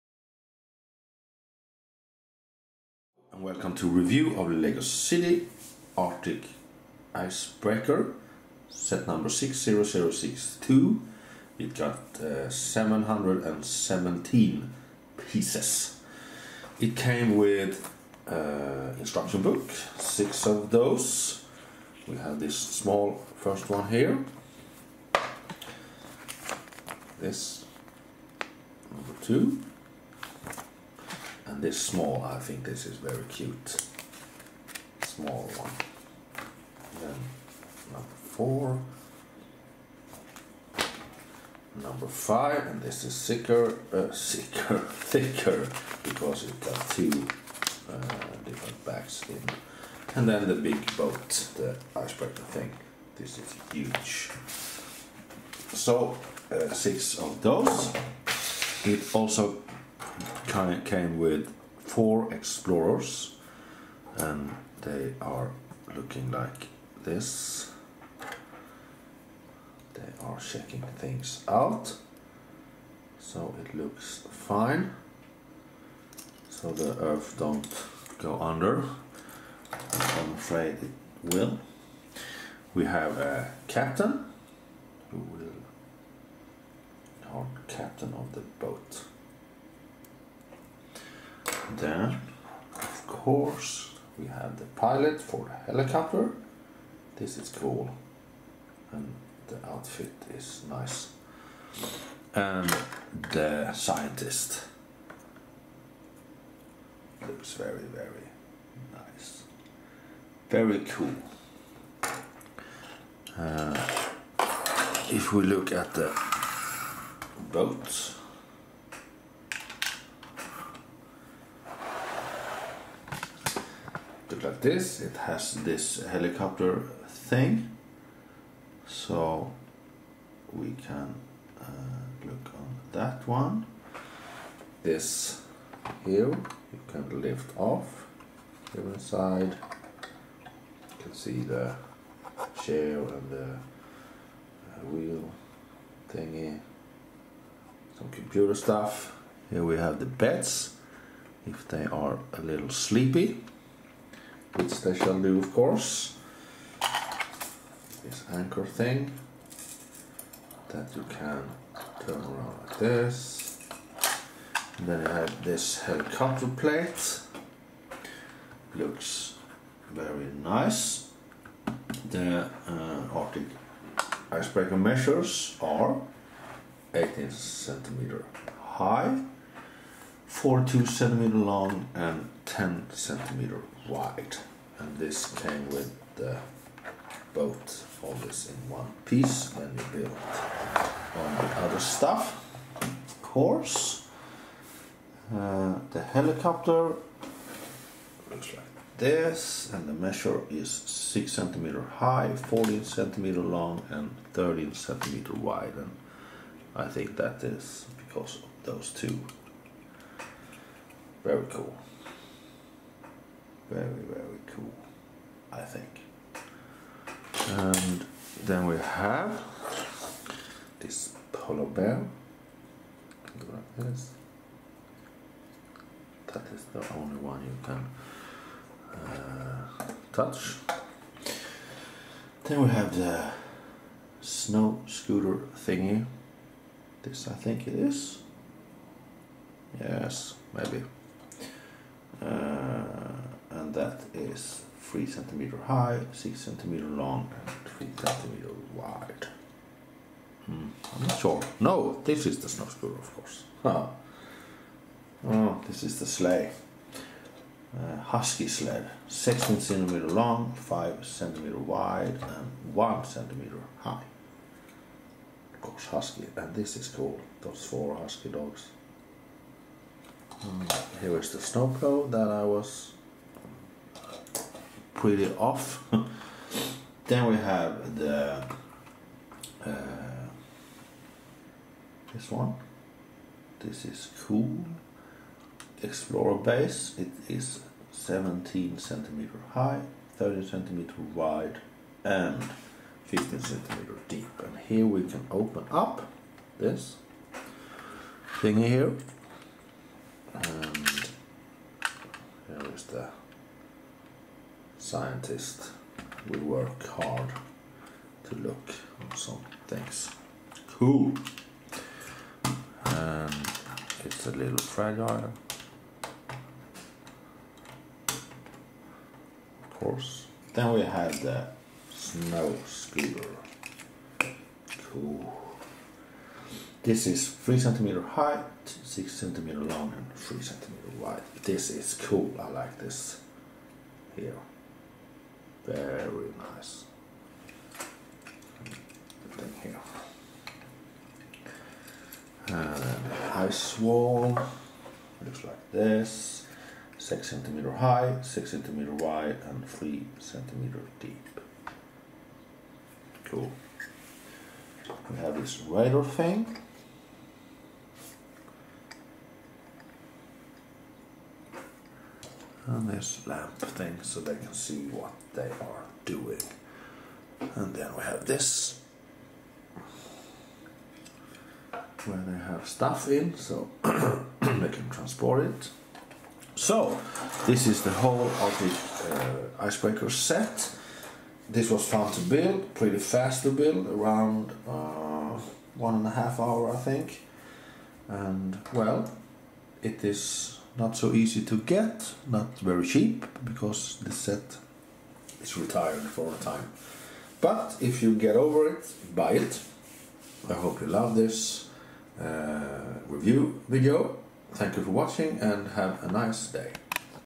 Review Lego City Arctic Icebreaker SET 60062 4K
Includes 7 minifigures with accessories: 4 explorers, captain, pilot and a scientist
Arctic Icebreaker features an anchor with winch and chain, snow scooter, removable bridge with steering wheel, windows and steps, 2 bunk beds, movable ladders, 2 cargo containers with equipment and a helipad
Helicopter features a cockpit for a minifigure, turning rotors, turning rear propeller and a winch function
Explorer’s base features an opening door and walls, radar and antenna, laboratory with desk, microscope, fridge, bed, computer and a ladder
Snow plough features rubber tracks, large front shovel, hook and chain, chainsaw and a walkie-talkie
Husky sled team features 4 huskies, a sled, camera, binoculars, pickaxe and a walkie-talkie
Also includes a polar bear, large antenna with gas cylinder and box, pylon, a fish, 3 translucent blue ice blocks and 5 crystal elements
Accessories include Arctic explorer hoods, a shovel, broom, magnifying glass, grinder, scanner and snowshoes
Turn the wheel to raise or lower the anchor
Launch the helicopter from the helipad and winch the cargo containers ashore
Go on research expeditions with the husky sled team
Break apart the ice wall or pull ice blocks with the snow plough’s hook and chain
Examine the mysterious crystals in the laboratory
Arctic Icebreaker measures over 7” (18cm) high, 16” (42cm) long and 4” (10cm) wide
Helicopter measures over 2” (6cm) high, 5” (14cm) long and 5” (13cm) wide
Snow scooter measures over 1” (3cm) high, 2” (6cm) long and 1” (3cm) wide
 Explorer’s base measures over 6” (17cm) high, 5” (13cm) wide and 5” (15cm) deep
Snow plough measures over 2” (6cm) high, 4” (10cm) long and 2” (6cm) wide
Husky sled measures over 6” (16cm) long, 1” (5cm) wide and under 1” (2cm) high
Ice wall measures over 2” (6cm) high, 5” (6cm) wide and 5” (3cm) deep